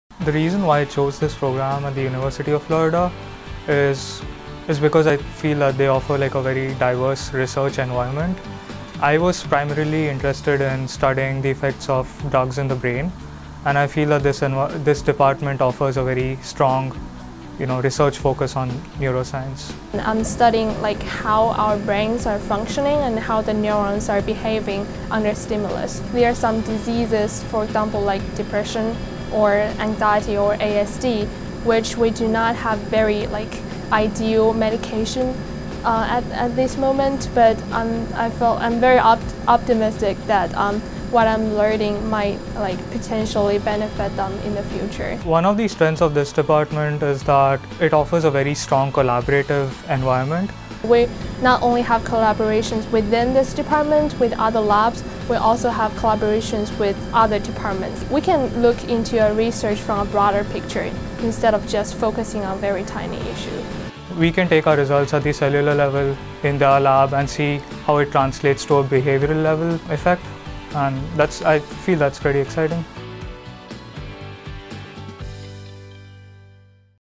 Sarthak Singhal and Yalun Tan University of Florida College of Pharmacy
The department of pharmacodynamics at the University of Florida College of Pharmacy offers graduate students a collaborative environment for students to pursue their interests in the neuroscience field. Sarthak Singhal and Yalun Tan are third-year students pursuing their doctorate degree. These young scientists are studying how the brain functions from a cellular and behavioral level. They are recognizing that success as a scientist depends upon the ideas and techniques that are shared amongst collaborators in their department and across disciplines at the University of Florida. For more information on the department of pharmacodynamics,